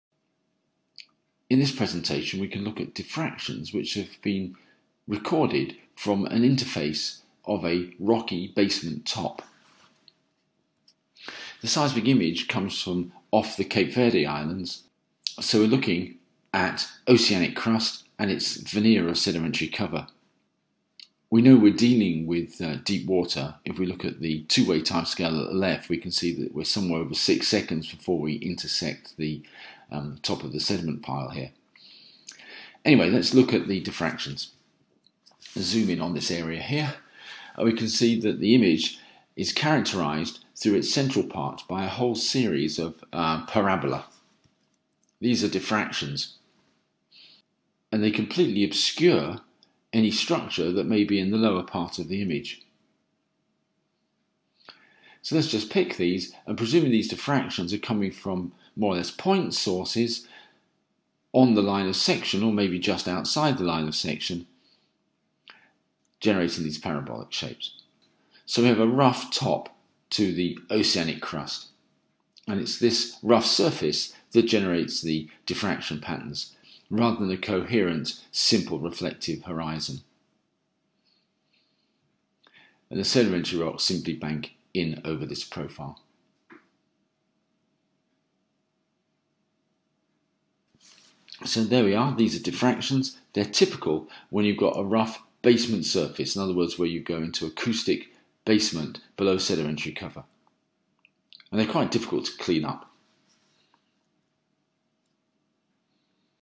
Diffractions
Part of The Shear Zone channel, the collection on introductory seismic interpretation exercises. This is a discussion of a profile from off the Cape Verde islands by BIRPS (from the Virtual Seismic Atlas). The exercise is available on the Shear Zone web site. It looks at diffractions.